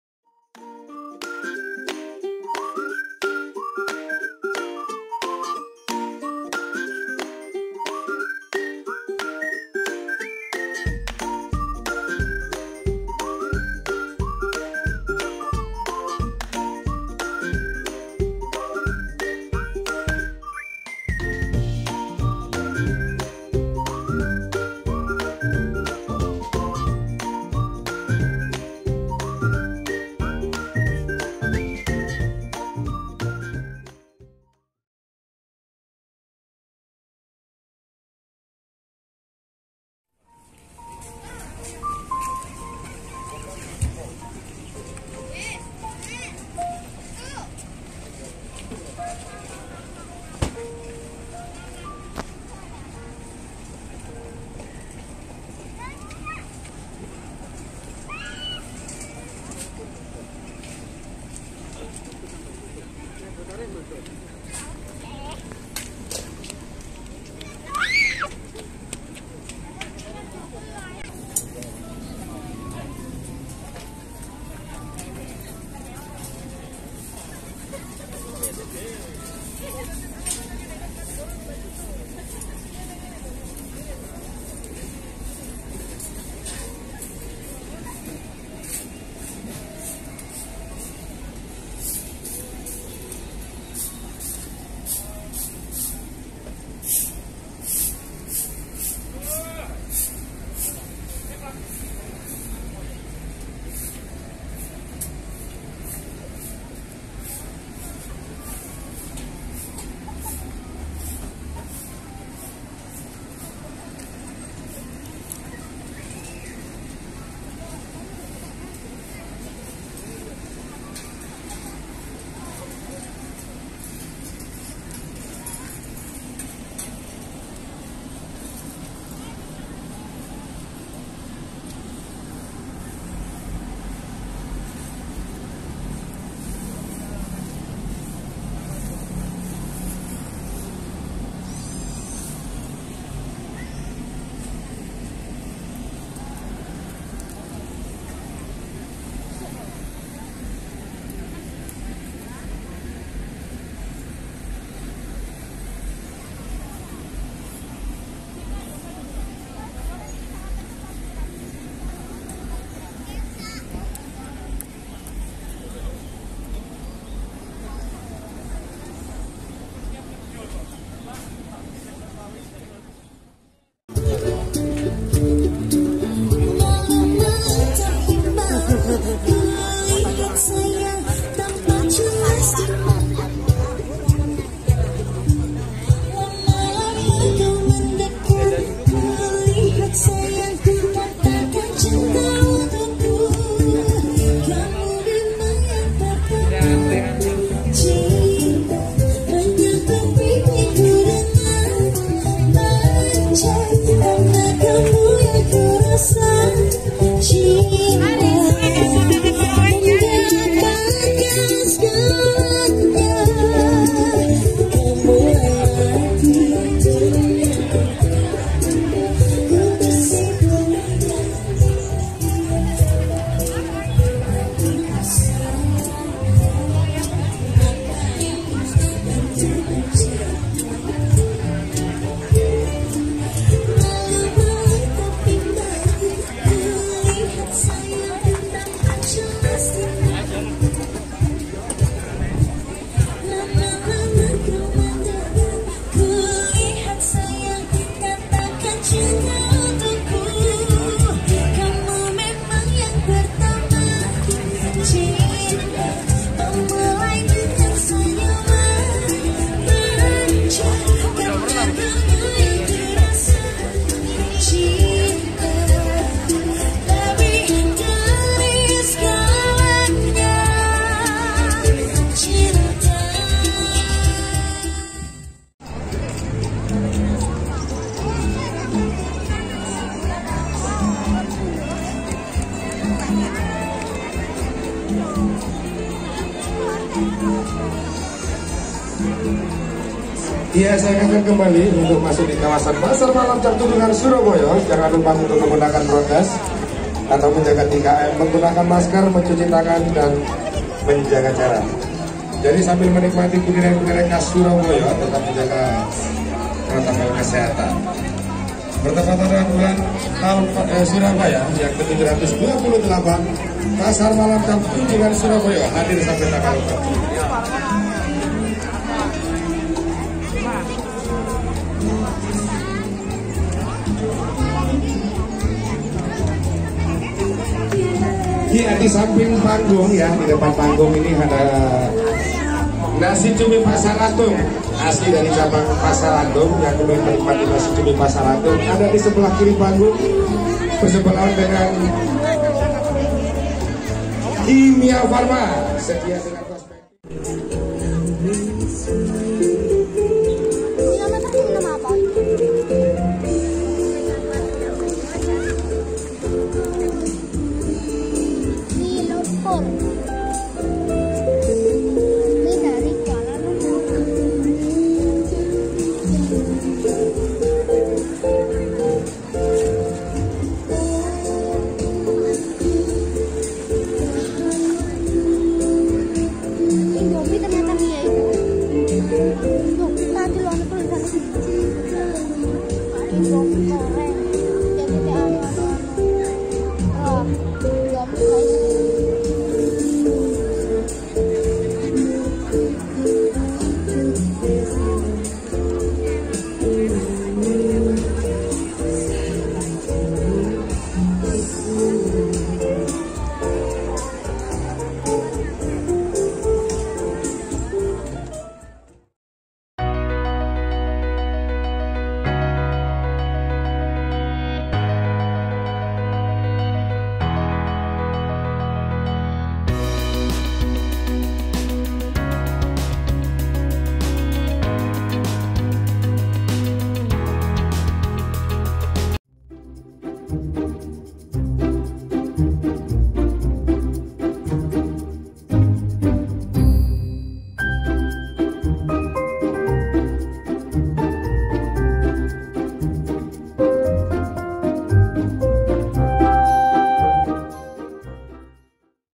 Pasar Malem Tjap Toendjoengan Suroboyo (Pakuwon City Surabaya).
Pasar Malem Tjap Toendjoengan adalah events kulineran setiap satu tahun sekali saat menyambut ulang tahun kota Surabaya.

tapi tahun 2020 event ini tidak dapat diselenggarakan karena pandemi covid 19.

Tahun ini pun saya kira tidak diadakan.
Ternyata Tepat tgl 31 Mei di buka kembali sampai tanggal 4 Juli 2021.

biasanya event ini diselenggarakan awal Mei hingga awal Juni.

Jadi Kawan-Kawan semua yg ingin tahu apa saja stand kulineran yg ada, silahkan meluncur ke Parkiran Pakuwon City Surabaya.

Selamat Berkulineeeeeerrrrr!

God Bless.